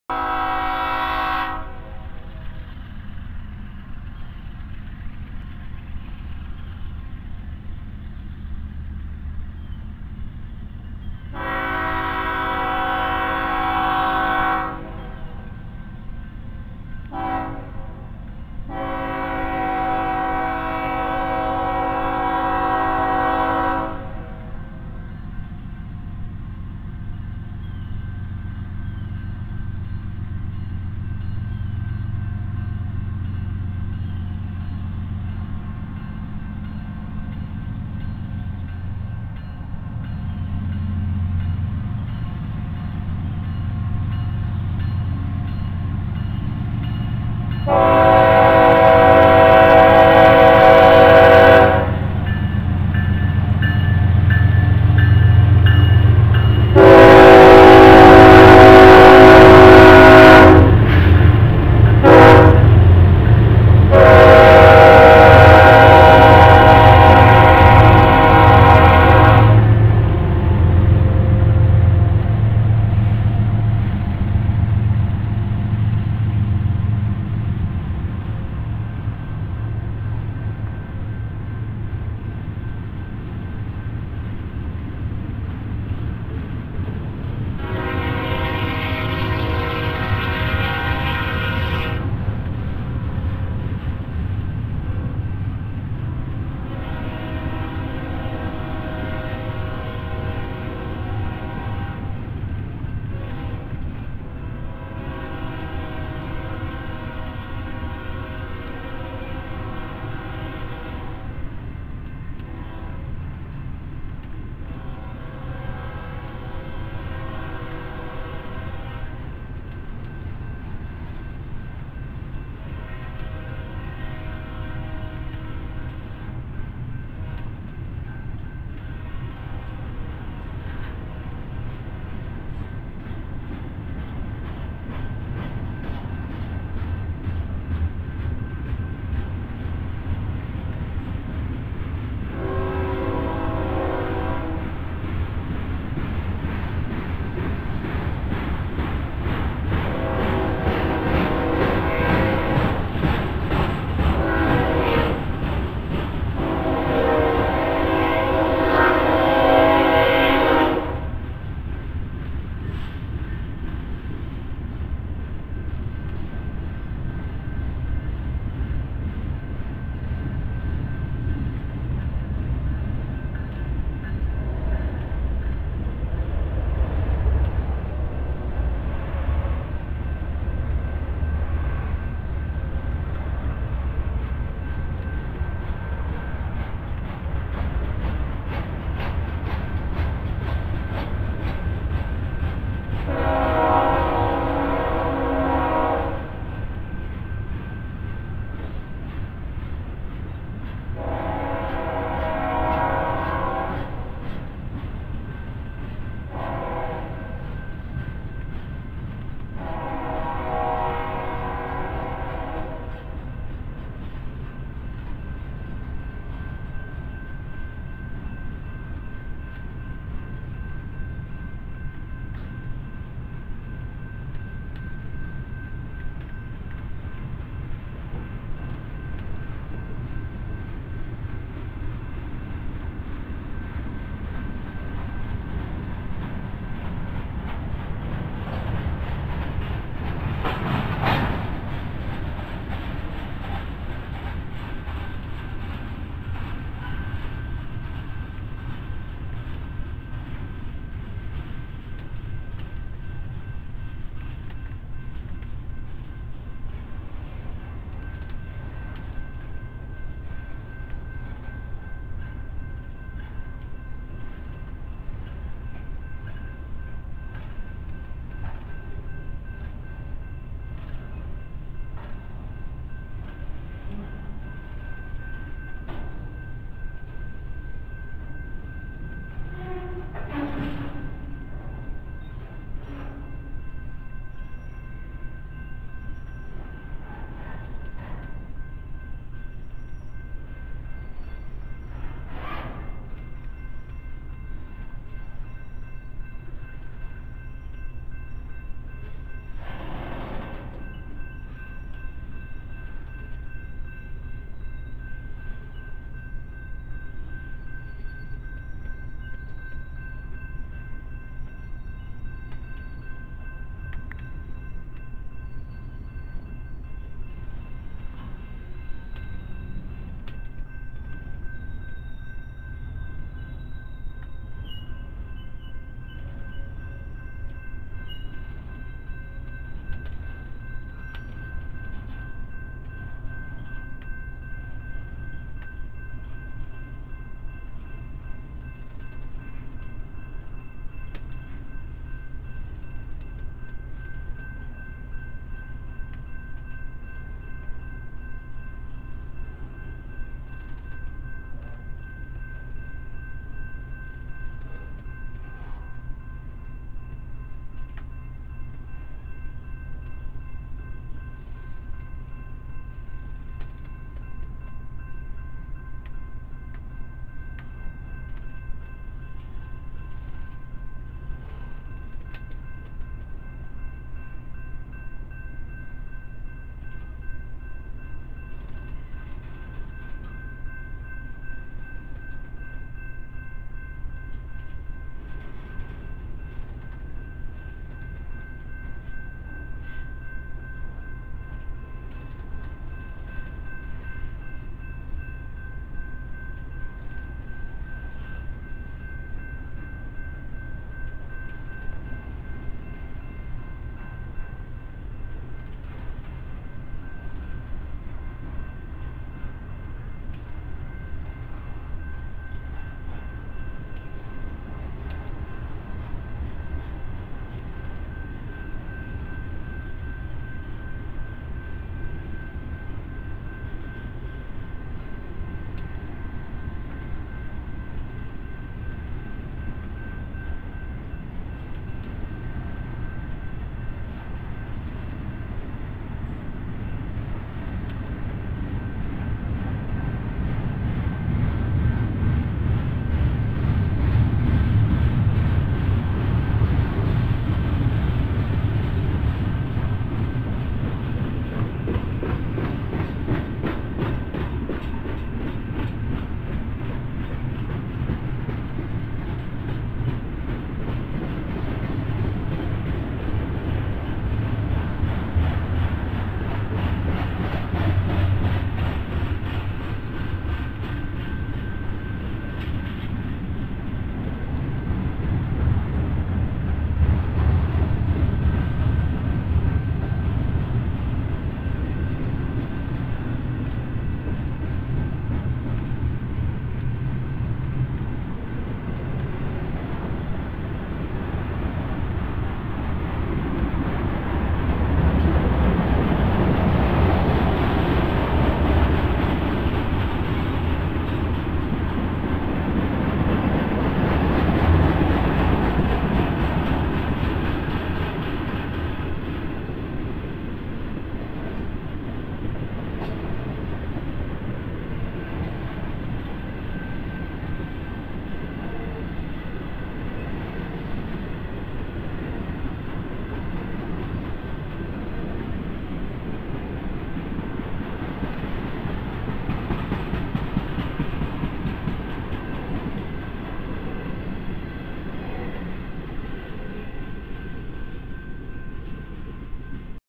VID201909038 BNSF 8276 ES44C4 northbound 14 September 2019 05 27 PM Edmond Oklahoma Ronald E Estes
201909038 BNSF 8276 ES44C4  northbound 14-September-2019 05:27 PM Edmond Oklahoma Ronald E Estes